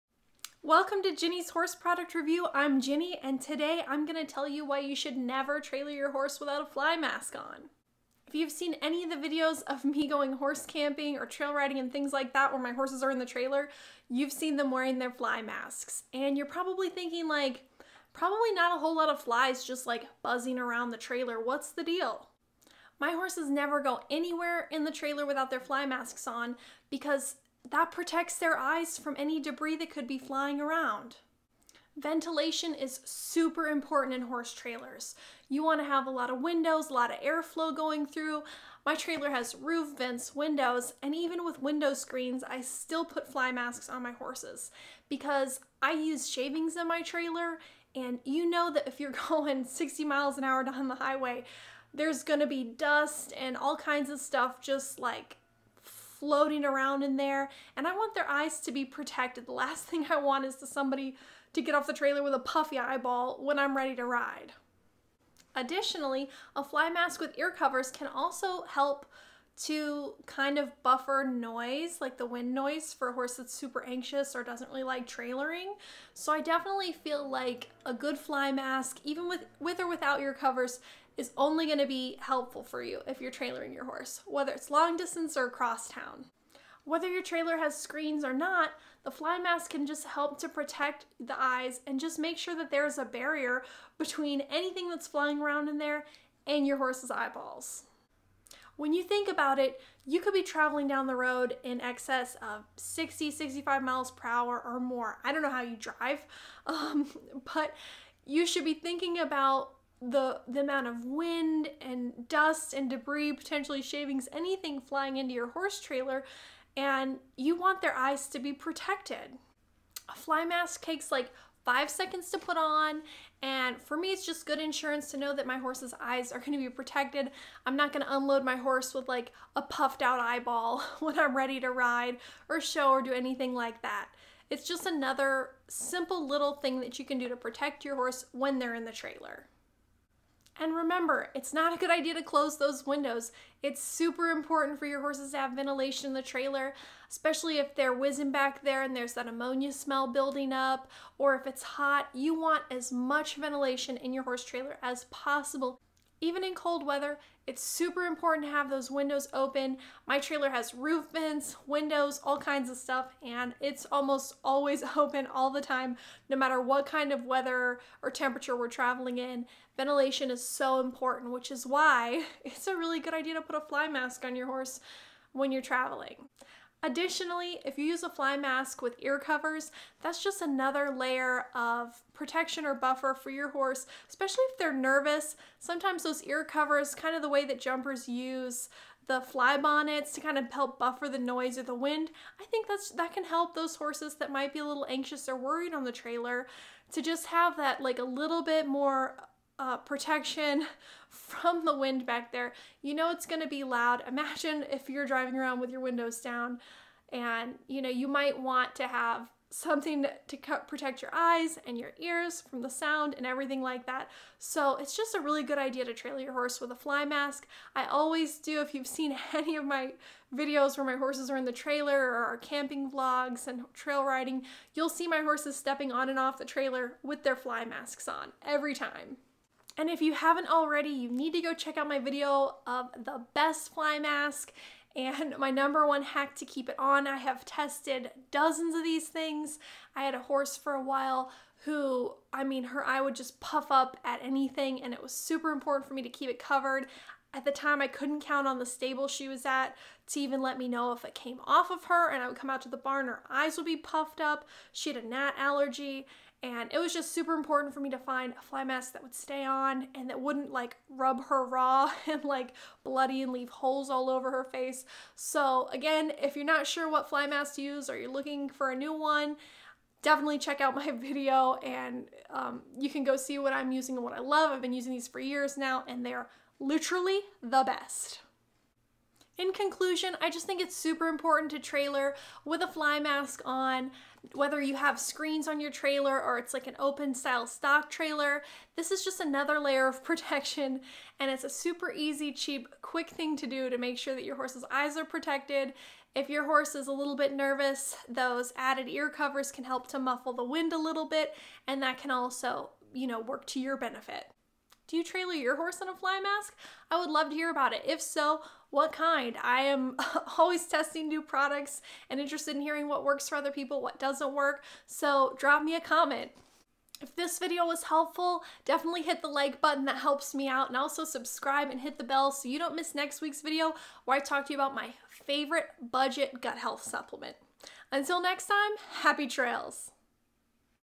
NEVER TRAILER WITHOUT A FLY MASK ON YOUR HORSE!
I always trailer my horses in fly masks, and you should too! Fly masks can provide protection for your horse's eyes from any debris that could be flying around back there. Even with window screens, it's important to put fly masks on before loading up your horse.

Additionally, a fly mask with ear covers may offer some noise protection in the form of wind-buffering for a nervous traveler.

My favorite fly mask is the mesh bug eye style, and you can get them on amazon!

Also, make sure to check out my blog about why this is my favorite fly mask, and the number one hack to keep it on!